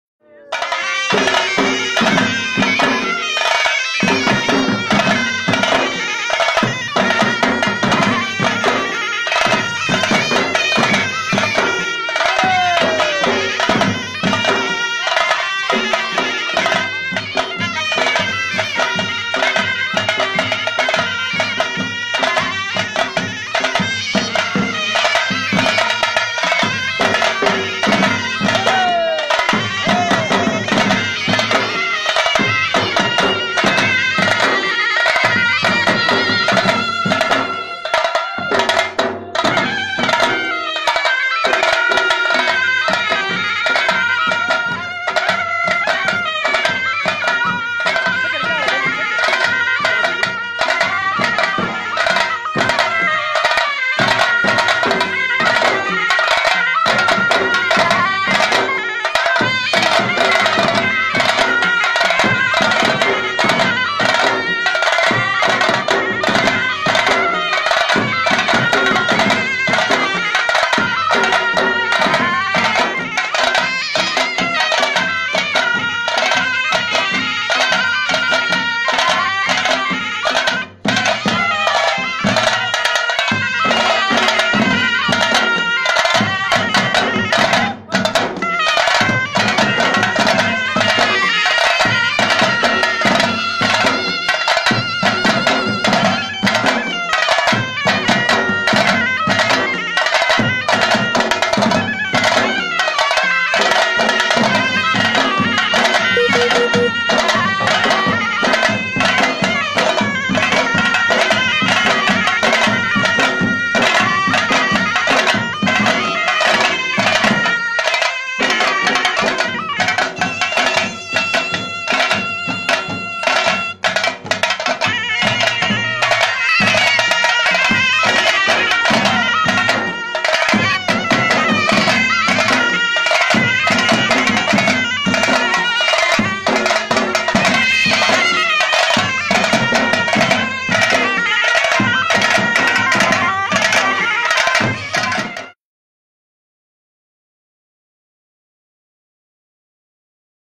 Hunza Hareep | Perfect Traditional Dancing skills | Gilgiti Dance
Musicians always  have the perfect dancing skills,HunzaOnline presents the the dance of two known traditional musicians of Hunza valley